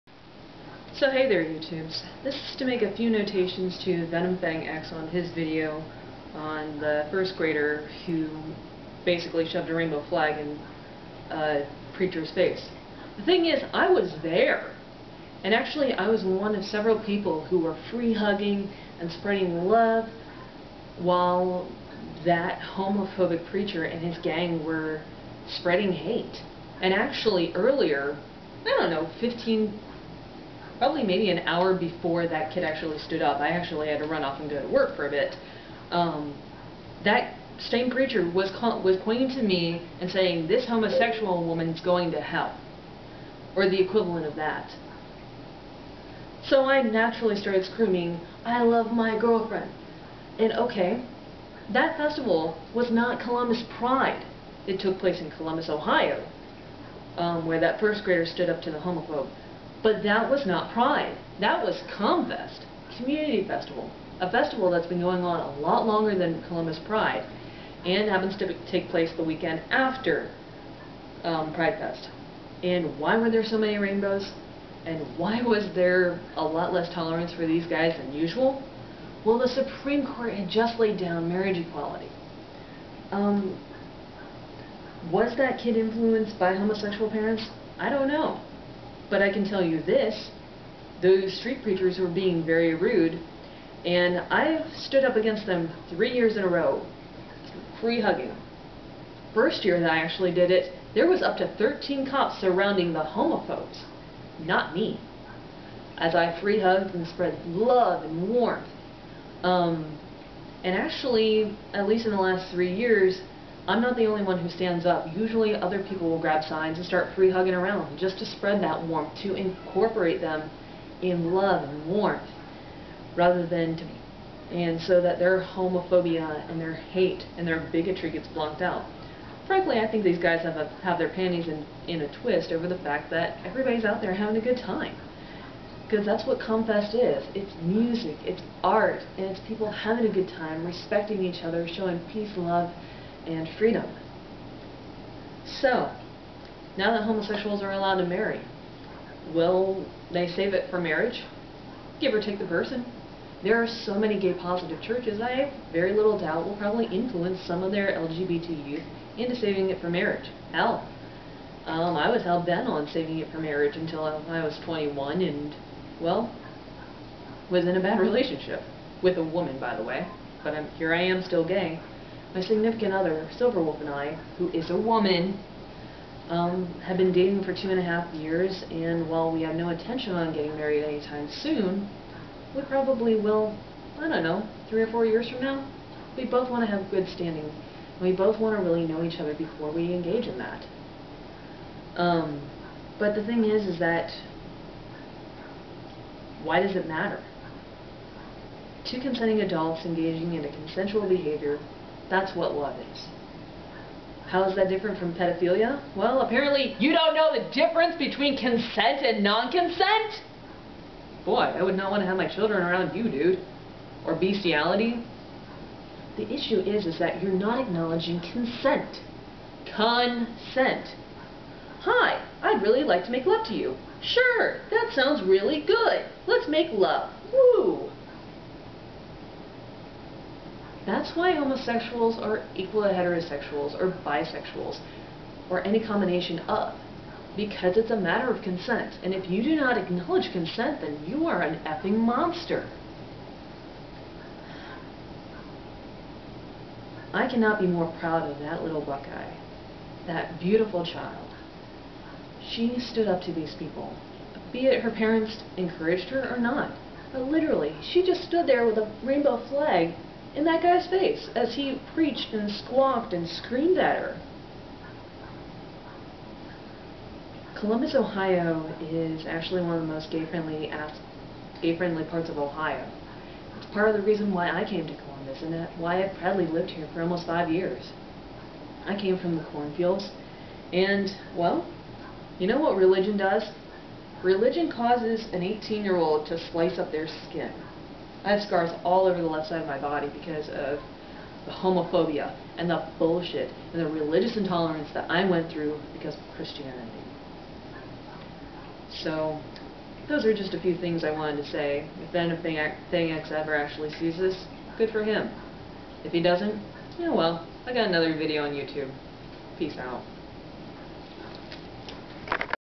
An Open Letter to VenomFangX ComFest 2015
This is an open letter to VenomFangX regarding his video about 'First Grader Stands up to street preacher'.